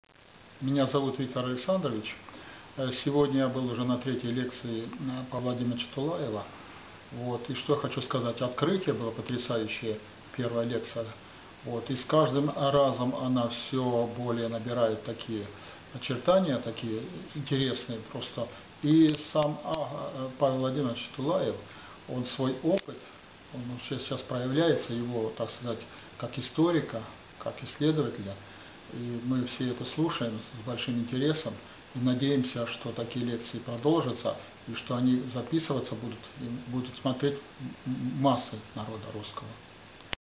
КРАТКИЙ ОТКЛИК ВИКТОРА АЛЕКСАНДРОВИЧА
Авторский курс Тулаева Павла Владимировича «РУССКИЙ МИР В ИСТОРИЧЕСКОМ РАЗВИТИИ»
Отзыв Виктора Александровича
"Открытие, 1-я лекция была потрясающая. И дальше интересно, сегодня 3-я лекция. Мы все слушаем с большим интересом".